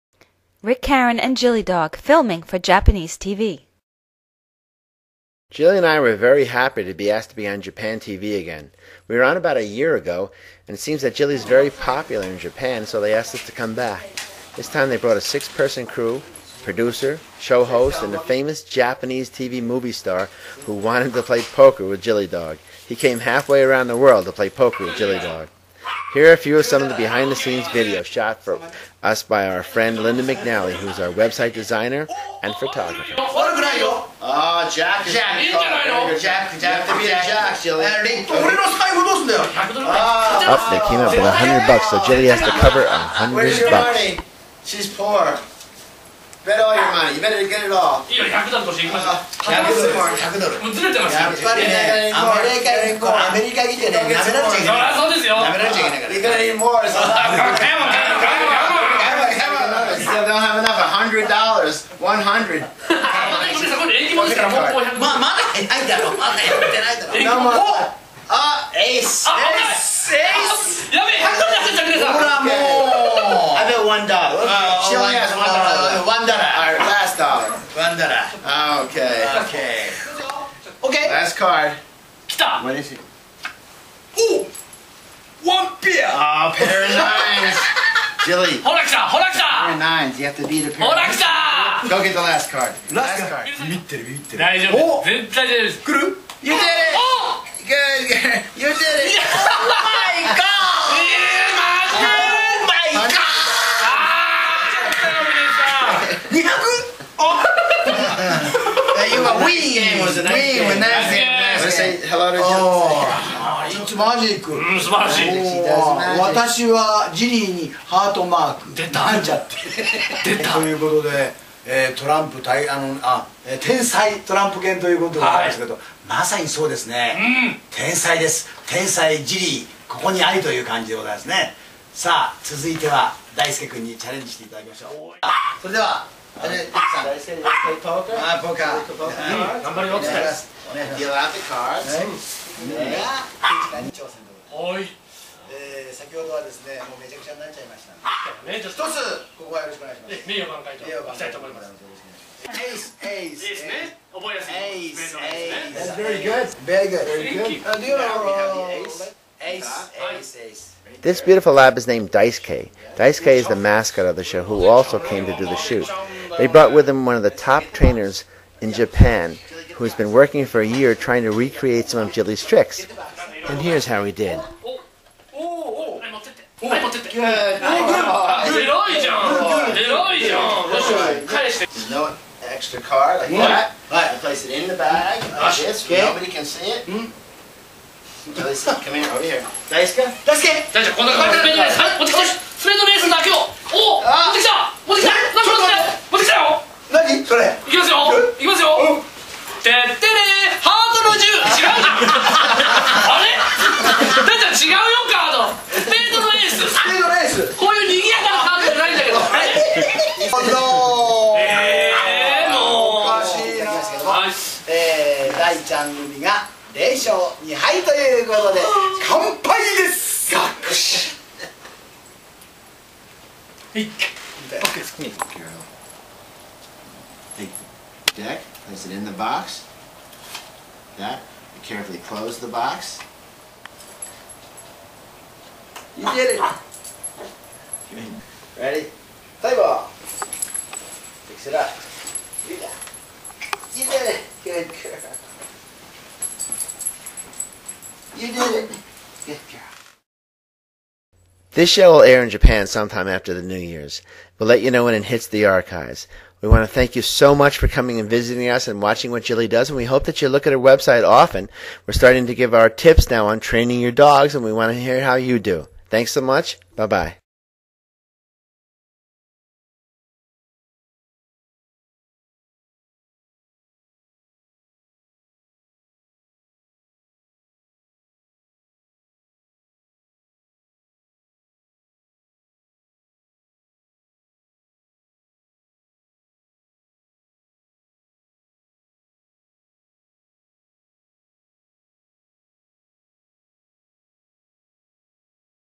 Rick Caran and Jilli Dog - Behind the scenes for Japan TV
Rick Caran and Jilli Dog - the world's only poker playing dog - filmed by Japanese Television for an appearance on the Pochi Tama show. Here are some behind the scenes from the shoot. We had a great time as you can see! for more and tips on training your best friend.